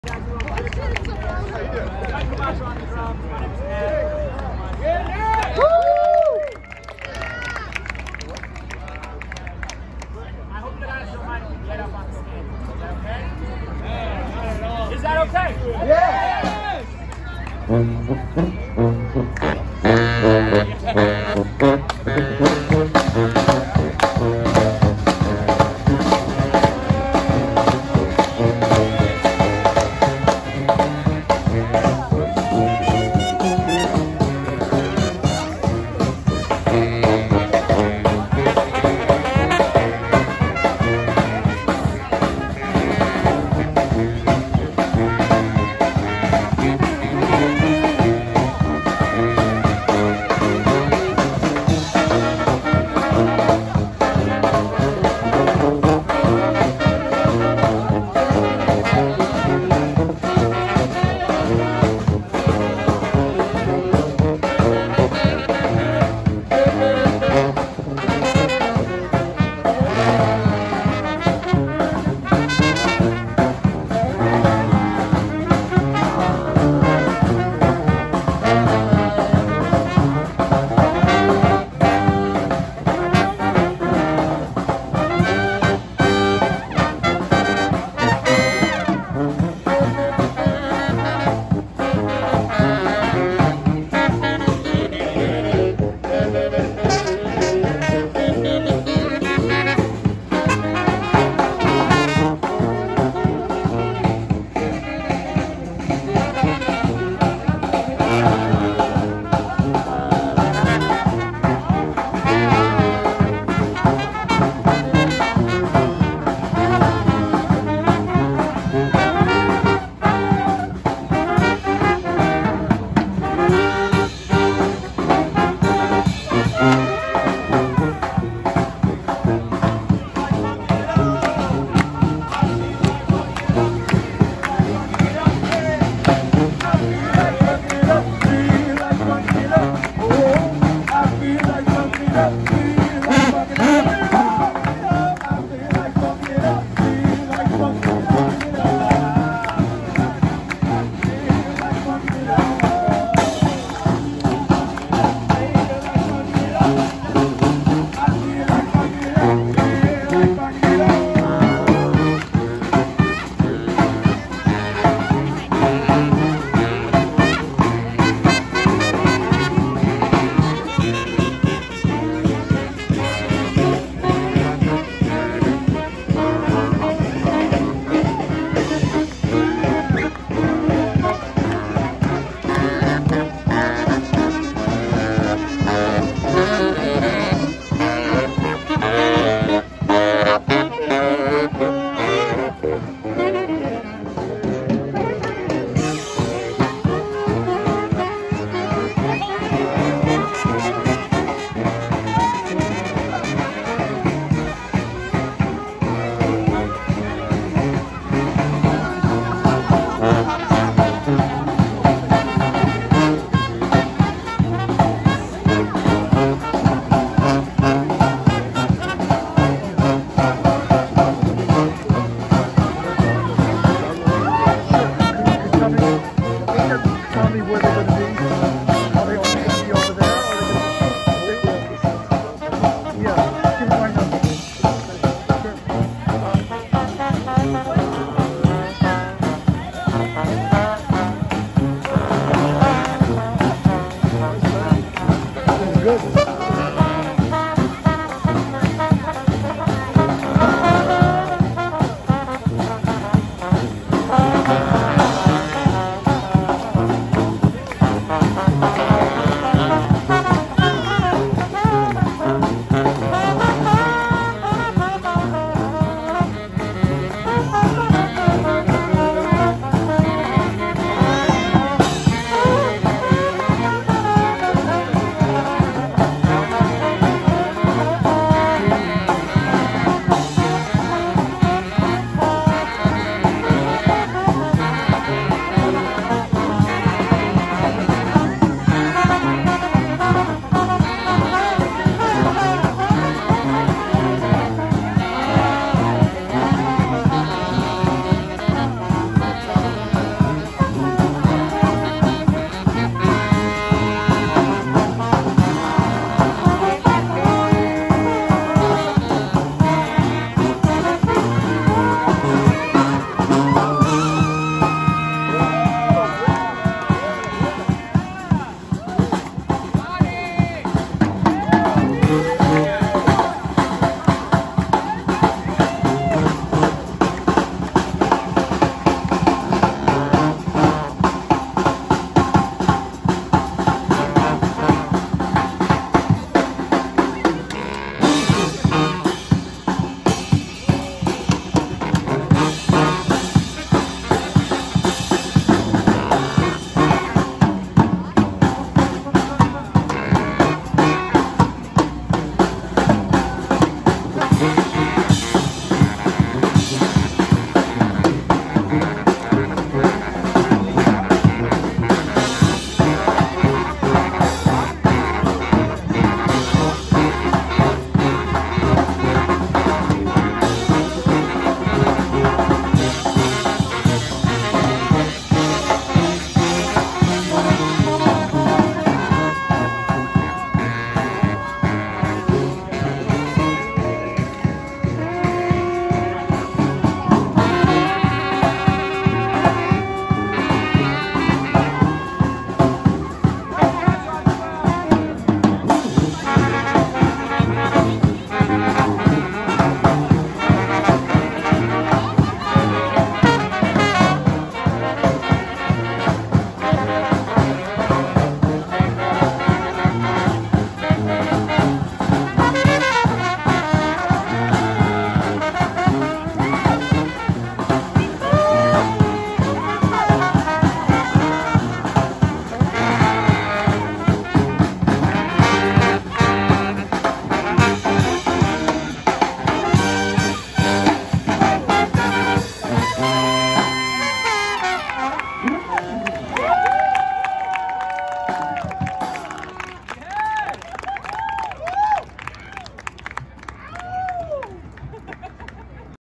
Roosevelt Island Day Celebration Concert With Music From Stoop Kidz Brass Band
June 12, 2021 Roosevelt Island Day Celebration with music from Stoop Kidz Brass Band at Mediation Steps on East River Waterfront (Part 1)